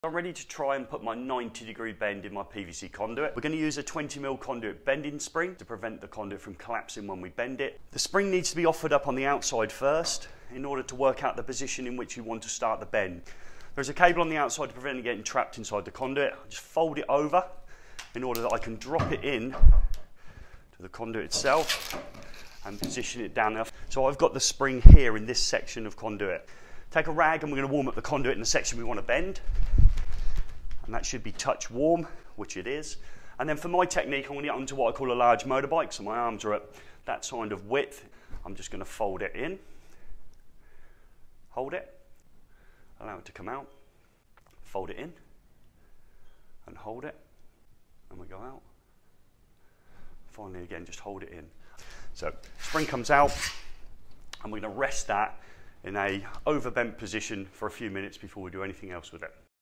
Do You Bend PVC Conduit Like This…?
How to Bend PVC Conduit to 90 Degrees 💪🏻 📐

In this short video I demonstrate my technique for putting a 90 degree bend in PVC conduit without bending the conduit over my knee. This method of bending PVC conduit make it easier to conform with the minimum bending radius of conduit as stated in BS 7671.

This video has been designed to go on the YouTube shelf as a short video in portrait mode.